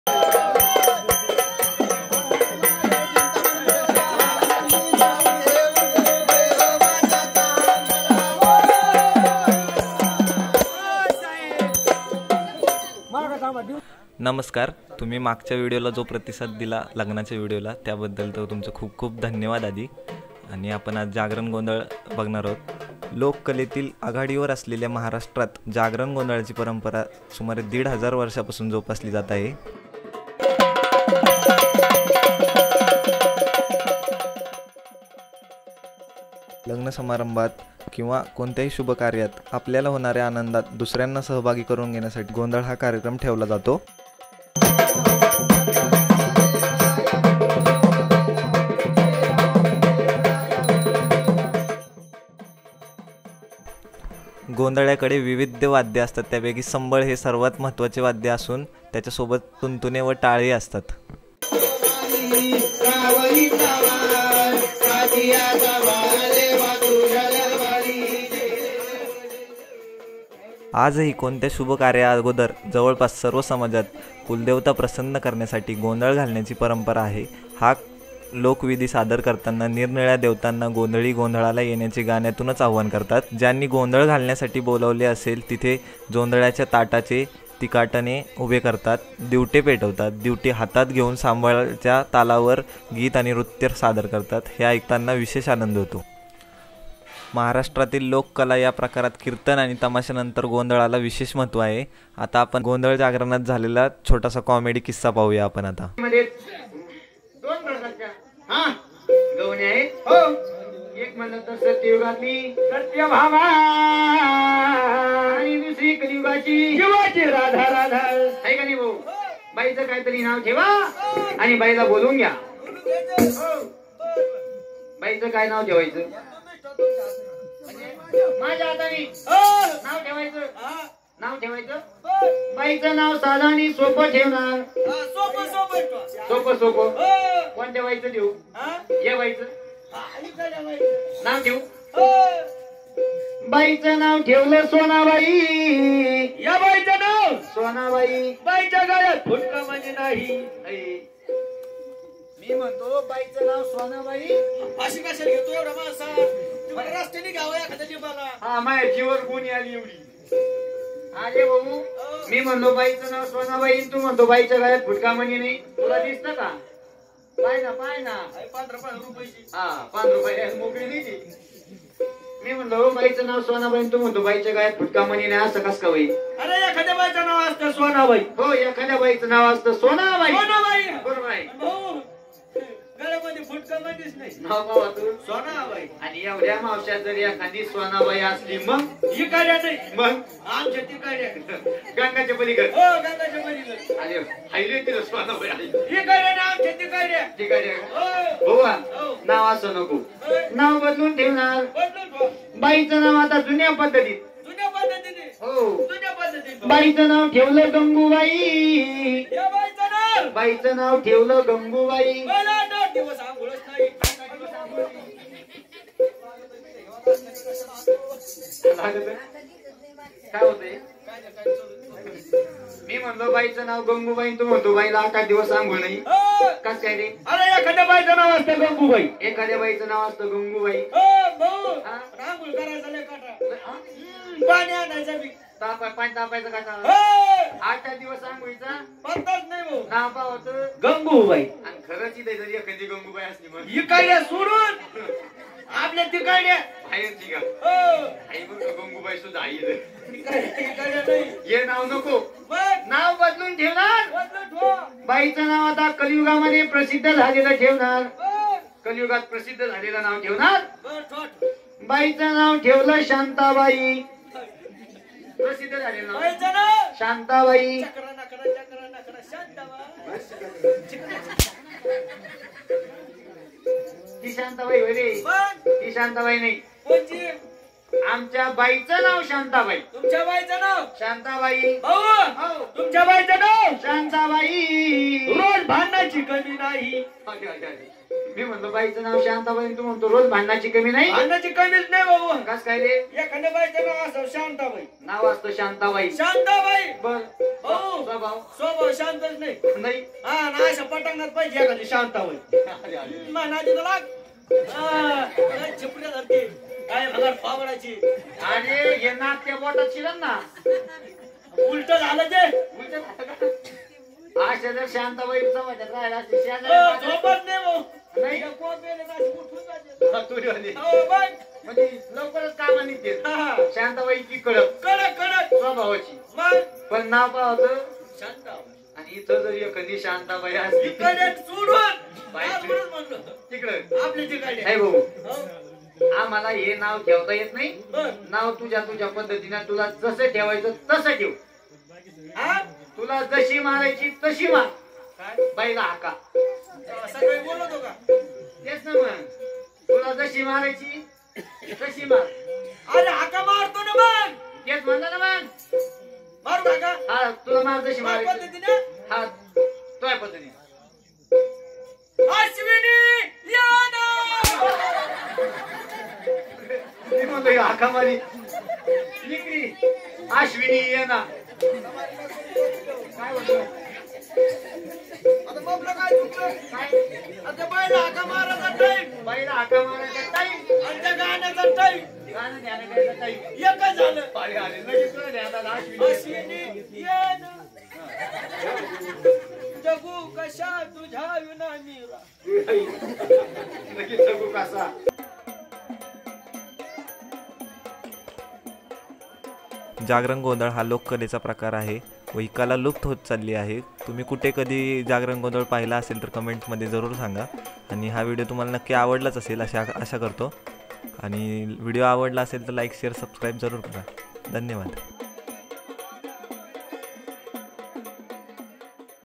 मराठमोळा पारंपरिक जागरण गोंधळ । Abhishek Baswekar
या कार्यक्रमामध्ये गोंधळी लोक सर्व देवतांची नावे घेतात. त्यांना गोंधळास येण्याचे आवाहन करतात. या कार्यक्रमामध्ये सर्वाना अन्नदान केले जाते.
महाराष्ट्रातील लोककला या प्रकारात कीर्तन आणि तमाशानंतर गोंधळाला विशेष महत्त्व आहे. गोंधळ आणि गोंधळी प्राचीन काळापासून लोककला प्रकारात मोडली गेले आहे. विशेषत: तुळजाभवानी आणि रेणुकामाता या देवींच्या संदर्भात गोंधळ घालण्याची पिढीजात परंपरा आहे. कालांतराने अन्य देवींच्या बाबतीतही गोंधळ घालण्याची प्रथा सुरू झाली.

अंगात तेलकट झबला, कपाळावर कुंकू, गळ्यात देवींची प्रतिमा व कवड्यांची माळ अशा वेशभूषेत आपल्या दारावर आलेली व्यक्ती म्हणजे गोंधळी. अंबेमातेच्या जयजयकार करीत ही व्यक्ती संबळ आणि तुणतुणे या वाद्यांच्या साहाय्याने विविध देवींचे गाणे सादर करतो. संबळाच्या तालावर नृत्यही करतो. त्याला 'संबळ गोंधळ' म्हणतात. तर काही गोंधळी काकडे पेटवून गोंधळ घालतात. ज्याला 'काकड्या गोंधळ' असे म्हटले जाते.

आजही कोणत्याही शुभकार्याअगोदर जवळपास सर्व समाजांत कुलदेवतेला प्रसन्न करण्यासाठी गोंधळ घालण्याची परंपरा आहे. हा लोकविधी सादर करताना निरनिराळ्या देवतांना गोंधळी गोंधळाला येण्याचे गाण्यातूनच आवाहन करतात. ज्यांनी गोंधळ घालण्यासाठी बोलावले असेल तेथे जोंधळ्याच्या ताटाचे तिकाटणे उभे करतात, दिवटे पेटवतात. दिवटी हातात घेऊन संबळाच्या तालावर गीत आणि नृत्य सादर करतात. हे ऐकताना विशेष आनंद होतो.